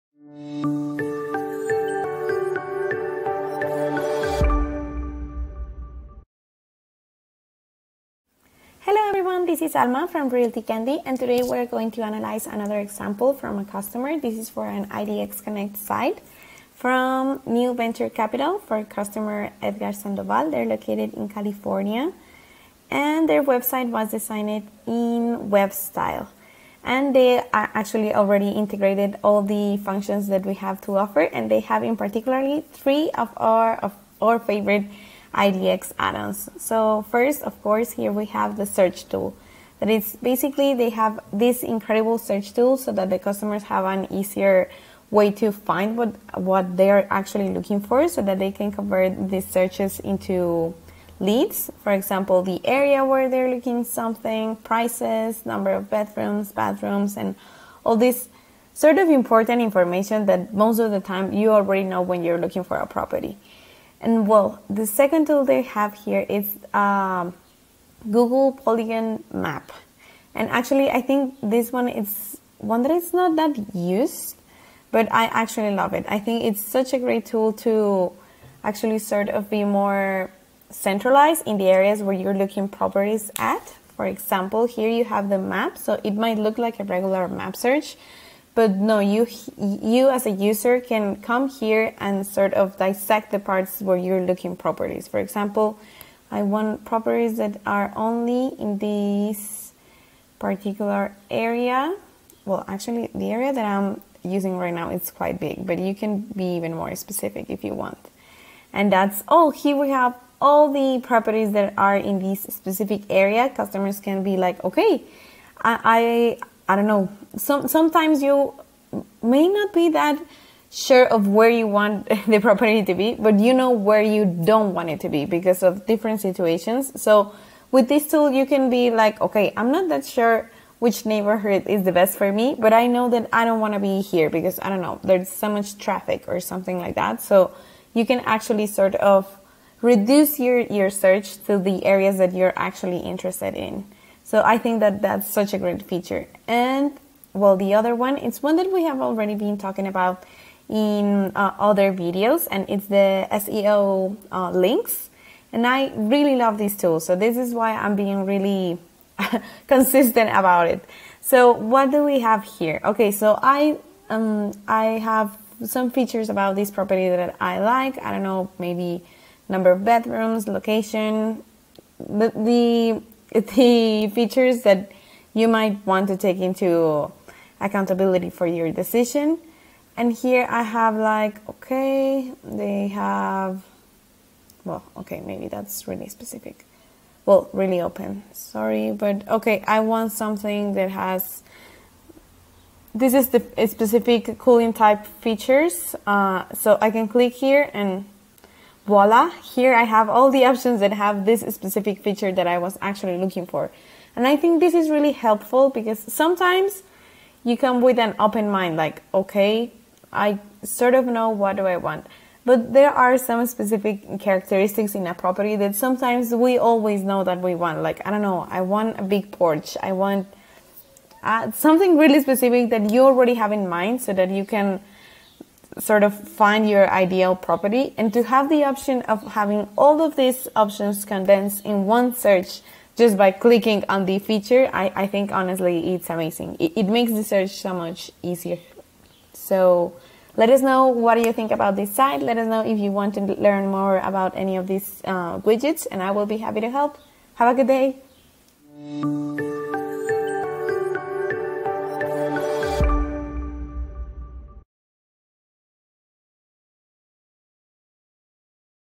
Mastering Google Map Polygon Addon for Property Searches Like Never Before! | New Venture Capital
IDXBroker | Unlock the power of Google Map Polygon Add-On to precisely define your property search areas! In this video, we dive deep into how to utilize this invaluable tool to create customized boundaries, making it easier than ever to find the perfect location for your properties. Learn step-by-step how to draw polygons, set specific parameters, and enhance your user experience. Say goodbye to endless scrolling and hello to efficient, targeted searches. Watch now and revolutionize the way you find properties!

Platform- Webstyle
Agency- New Venture Capital
City- Riverside, CA